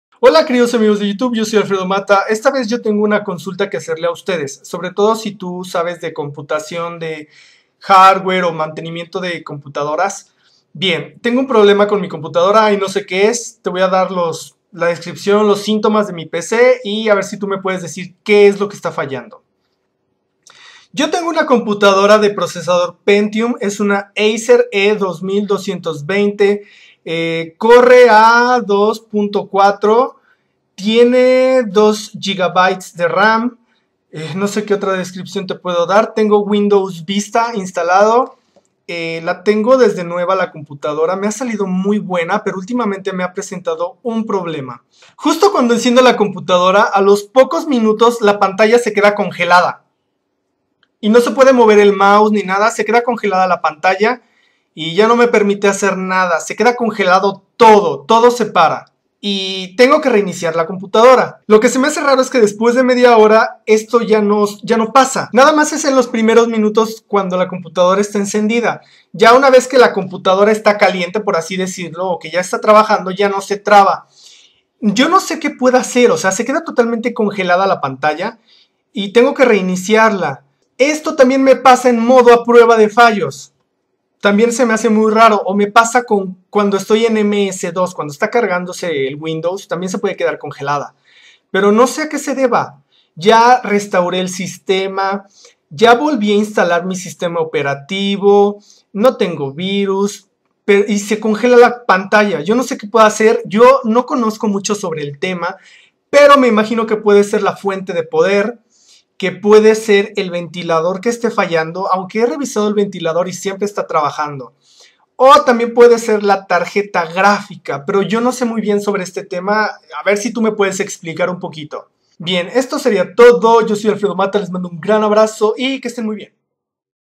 ¿Que tiene mi computadora?
Hola chicos, si alguien que sabe de computadoras me puede ayudar...
Mi PC es una Acer E2220
Procesador Intel Pentium Core Duo
Corre a 2.4 Ghz
Tengo 2 GB en RAM
Tengo 320 GB de espacio en disco (solo he ocupado 70 GB en total)
Tarjeta Gráfica en Placa NVIDIA® GeForce® 7100
Tengo 5 años con ella y nunca ha visitado al Técnico (nunca me ha dado problemas)
Con Windows Vista (así venia de fabrica)
Solo yo uso esa PC, mis familiares tienen sus respectivos ordenadores.

Solo la ocupo para mi trabajo (documentos) y para los vídeos, no tengo instalado nada, tengo el AVAST anti virus y me ha salido fenomenal.

El ordenador me ha salido fenomenal, no tengo quejas, solo que hace un par de días, EN LOS PRIMEROS MINUTOS AL ENCENDER LA COMPUTADORA... SE CONGELA LA PANTALLA... Así se queda "paralizada" y la tengo que reiniciar.

Solo sucede la primera vez que la prendo en  el día... Ya una vez trabajando, después de 15 - 30 minutos, lo deja de hacer... Como si al calentarse dejara de producir este error y al estar fría lo comete...

Ya se ha quedado pegada en MODO A PRUEBA DE FALLOS y en MS- DOS... Ya reinicie el sistema (punto de restauración) e incluso volví a instalar el Windows y me da el mismo error...

¿Alguien me puede orientar que es lo que puede estar fallando?

Yo no tengo mucho conocimiento sobre estos temas, pero pienso que puede ser:

- La fuente de poder
- El ventilador
- Tarjeta gráfica

* PIDO EL CONSEJO DE USTEDES, PORQUE NO QUIERO IR DESPREVENIDO AL TÉCNICO... Y QUE ESTE NO ME QUIERA ENGAÑAR.

Si alguien sabe de este tema y me pudiera orientar estaría agradecido =)

Como dato curioso les puedo decir que mi primer computadora fue una PENTIUM III en 1998 y dure con ella 10 años... Aun funciona muy bien! Así que a mi mis cosas me duran mucho tiempo y las cuido mucho =)

~ Alfredomatta